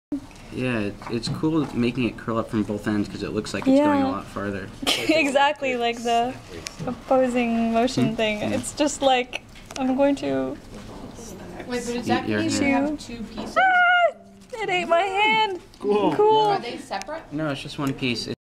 SMA mechanism: double curling
This mechanism uses two curling pieces moving opposite each other to create a claw-like motion.  The curling strip is made by sewing flexinol wire to the paper strip and then attaching the center of the strip to the base piece of paper.  When the wire heats up (from current running through), it contracts causing the paper to curl.

Made by Shaun Salzberg as part of the MIT/RISD paper-based electronics workshop.